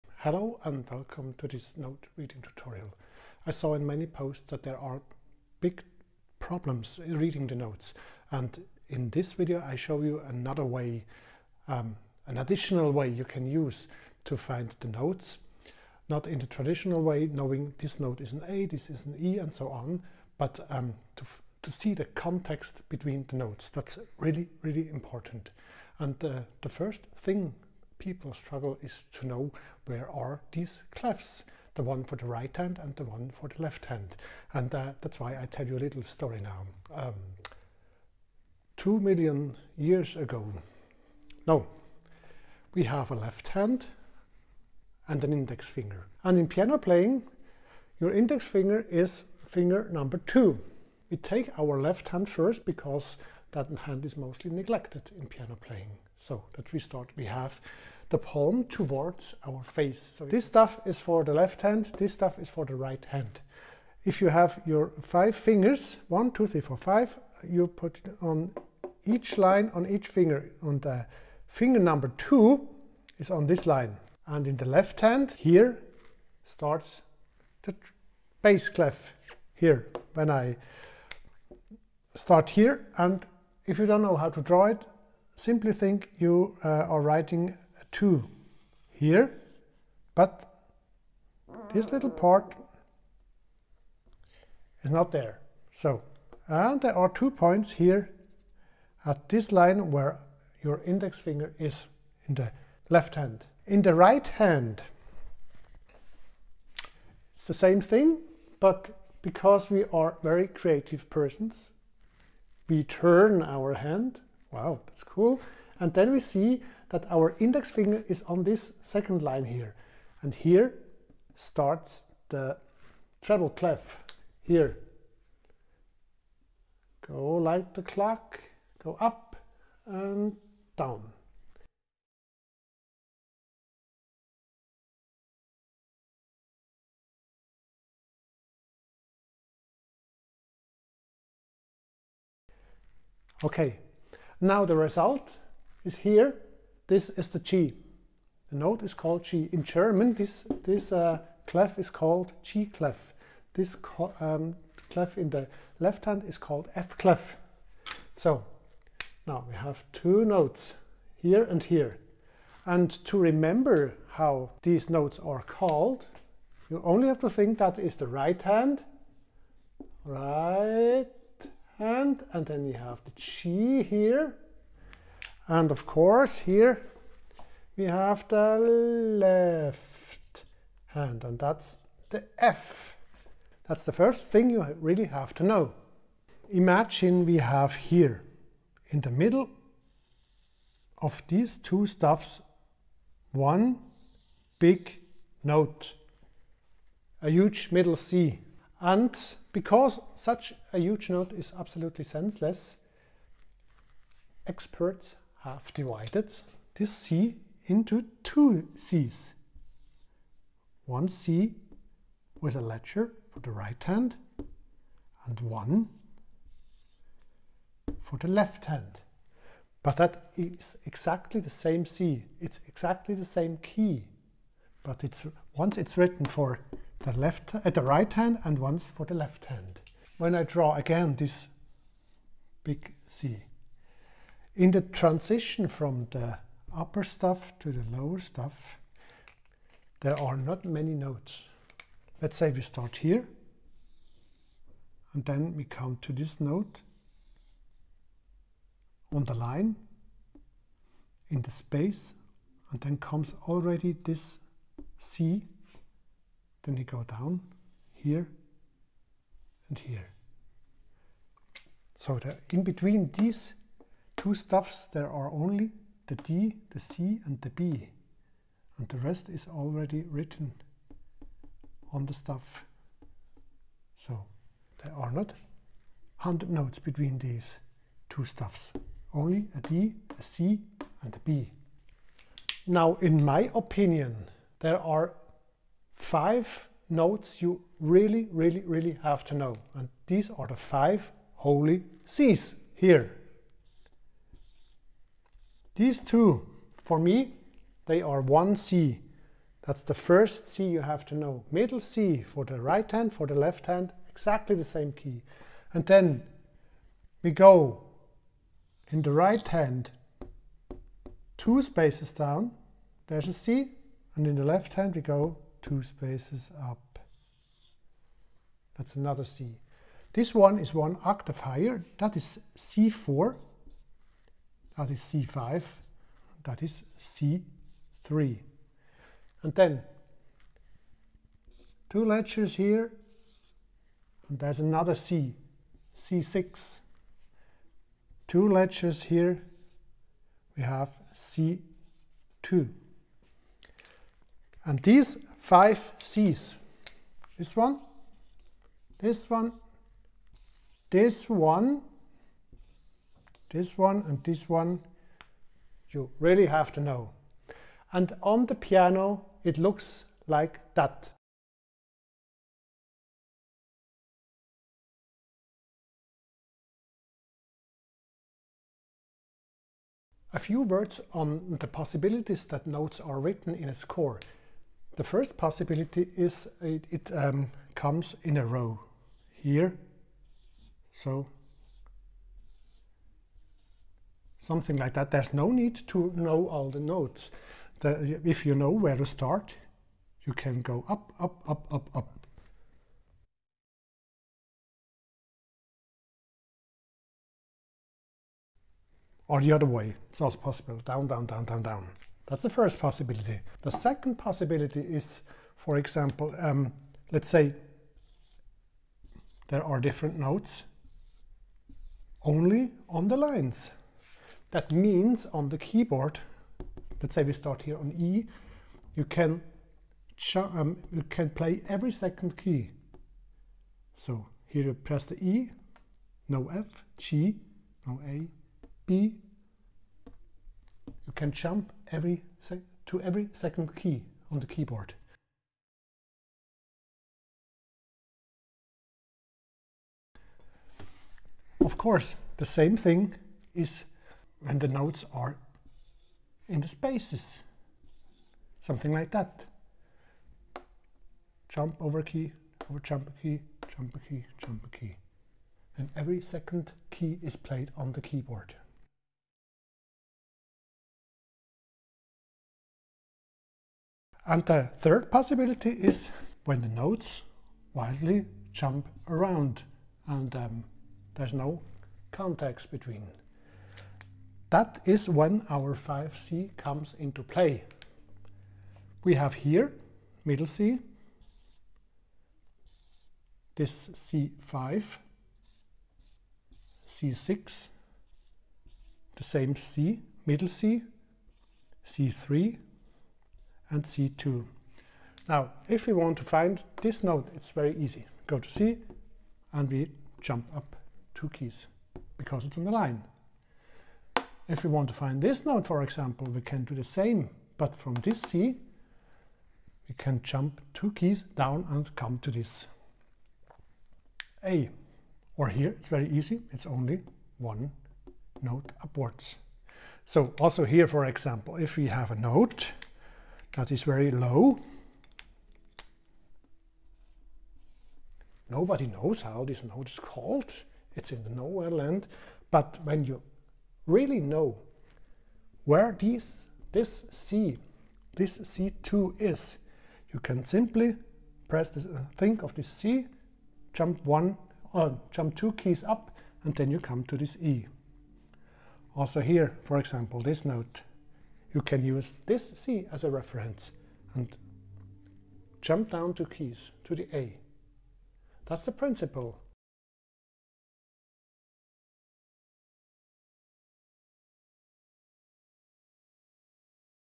How To Read Notes with Ease! An Alternative Way to The Traditional Notes Reading. Part 1
A simple explanation of how to read notes in an alternative way.